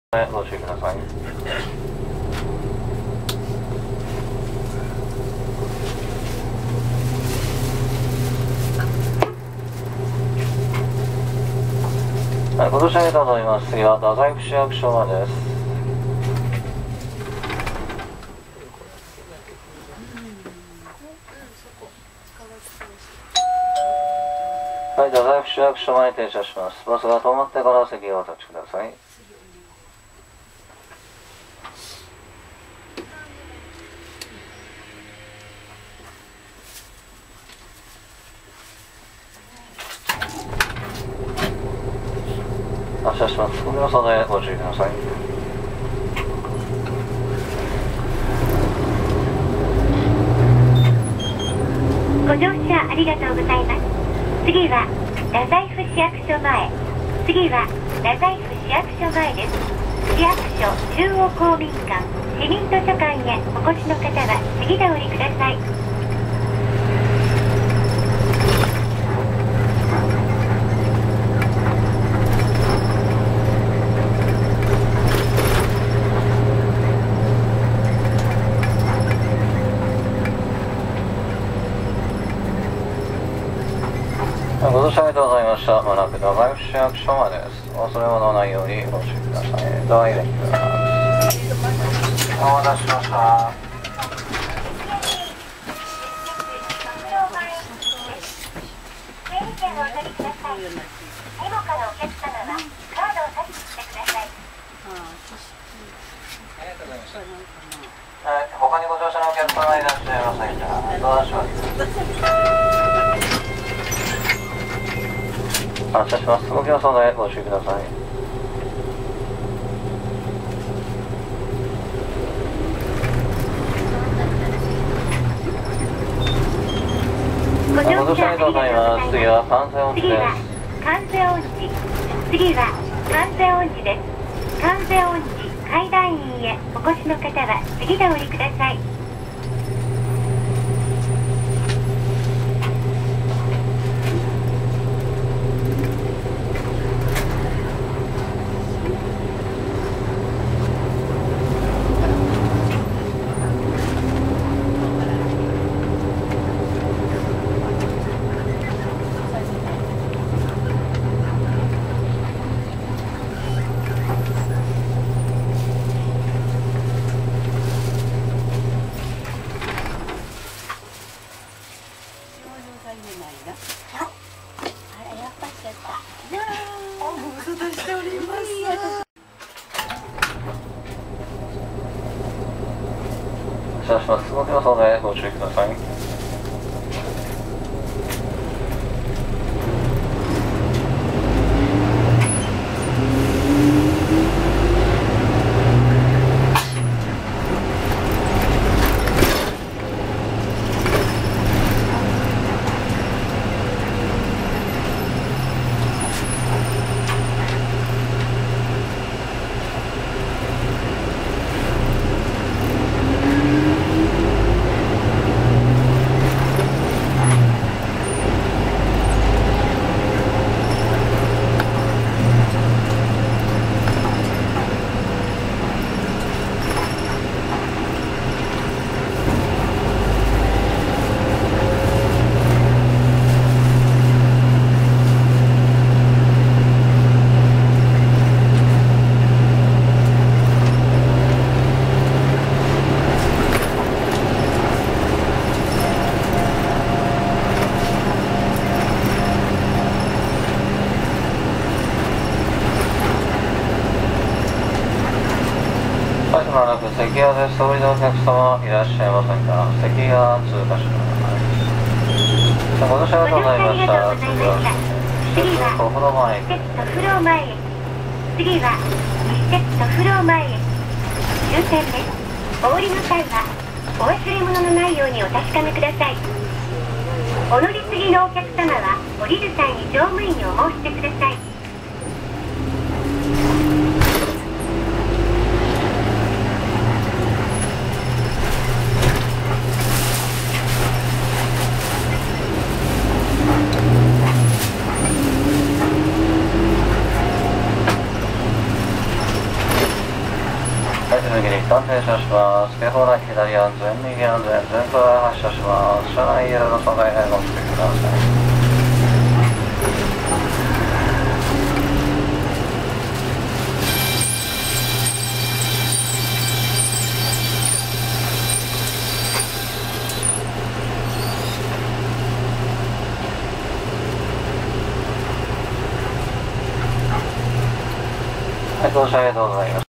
西鉄バス二日市5424号車まほろば号
太宰府市のコミュニティバスであるまほろば号です。車両は西鉄のRN　5424号車

社名:西鉄バス(西鉄バス二日市委託)
車種:KC-RN210CSN
車番:福岡200か･200
社番:5424
撮影日:2010/03/26